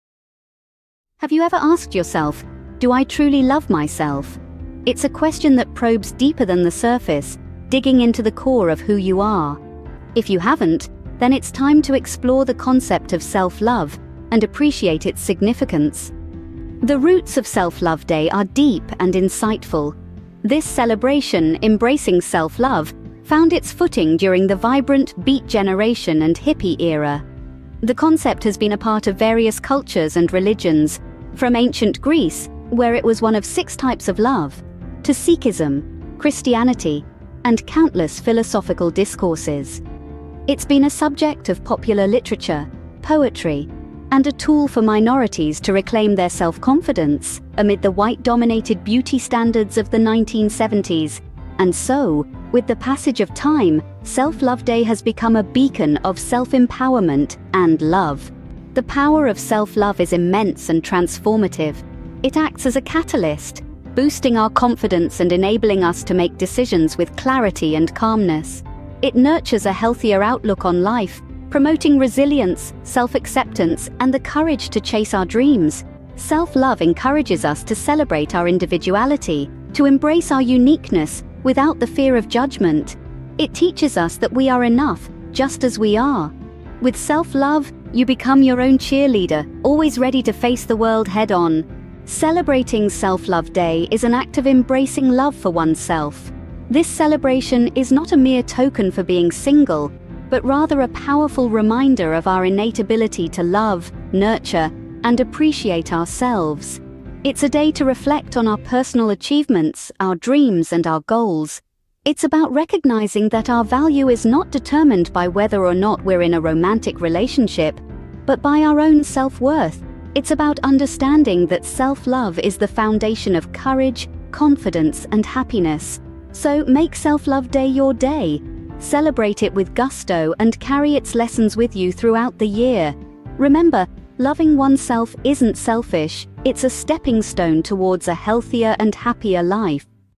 Celebrate Yourself: The Power of Self-Love
In celebration of Self-Love Day, join us as we delve into the power and importance of self-love. In this video, we explore the undeniable impact that self-love can have on our lives, backed by known facts.

Discover the benefits of embracing self-love, from increased confidence and empowerment to improved mental and emotional well-being. We'll explore various strategies and practical tips for cultivating self-love in your daily life, allowing you to become your own biggest advocate.

Join the conversation and share your own experiences with self-love in the comments below. Let's spread the message of self-acceptance and inspire others to embark on a journey of self-discovery.

Together, let's create a world where self-love is celebrated and nurtured.

OUTLINE:
00:00:00
Beginning with a Question

00:00:15
Unwrapping the History of Self-Love Day

00:00:57
The Power of Self-Love

00:01:31
Celebrating Self-Love Day